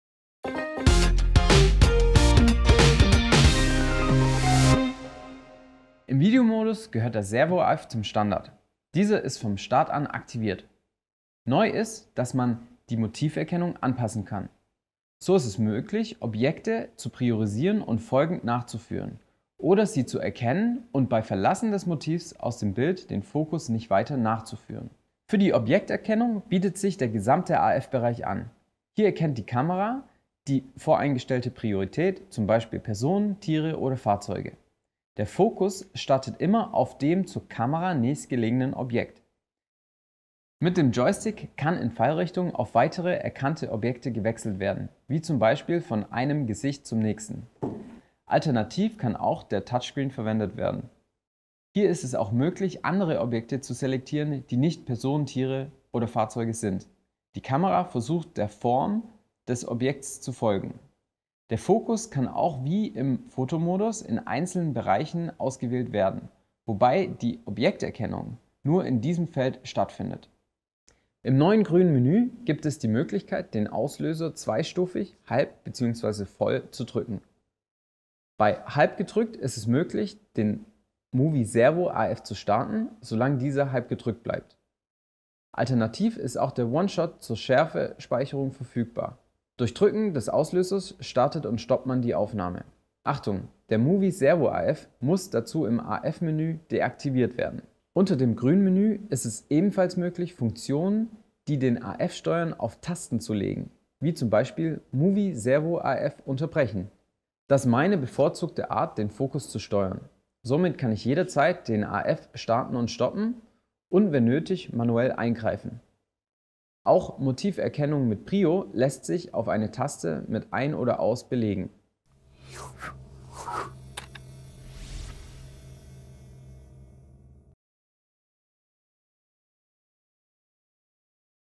EOS R1 & EOS R5 Mark II Profi-Feature: Video-Autofokus
In diesem Quick-Tipp erklärt Canon Ambassador und Academy Trainer Nicolai Deutsch wie du den Video-Autofokus konfigurieren kannst. Im Videomodus ist der AF-Modus Servo AF standardmäßig aktiviert. Canon EOS R1 und EOS R5 Mark II Kameras bieten erweiterte Funktionen für die Fokuskontrolle.

Mit Hilfe der Motiverkennung kannst du die Motiverkennung priorisieren, etwa auf Personen, Tiere oder Fahrzeuge. Fokussiert wird automatisch das nächstgelegene Objekt. Mit dem Joystick kannst du zwischen mehreren erkannten Objekten wechseln, z. B. von einer Person zur nächsten. Alternativ kannst du den Touchscreen verwenden, um Objekte zu selektieren, die nicht unter die drei Kategorien fallen. Der Servo AF folgt dann der Form des gewählten Objekts.
Die Objekterkennung lässt sich auch auf einen AF-Bereich eingrenzen, um den Fokus auf ein Motiv zu lenken.

Im grünen Kameramenü kannst du den Auslöser der Kameras zweistufig konfigurieren. Bei halb gedrücktem Auslöser startet der Movie Servo AF. Er bleibt aktiv, solange der Auslöser gehalten wird. Alternativ kannst du mit One-Shot-AF die Schärfe speichern. Drückst du den Auslöser durch, beginnt oder stoppt die Kamera die Aufnahme. Nico bevorzugt diese Möglichkeit der Fokuskontrolle, wenn der Movie Servo AF im Menü deaktiviert ist.

Für erweiterte Kontrolle lassen sich die AF-Funktionen auf bestimmte Tasten legen. So kannst du den Movie Servo AF unterbrechen, um manuell einzugreifen. Oder die Motiverkennung kann auf eine Taste gelegt werden, um sie mit einem Knopfdruck zu aktivieren oder zu deaktivieren.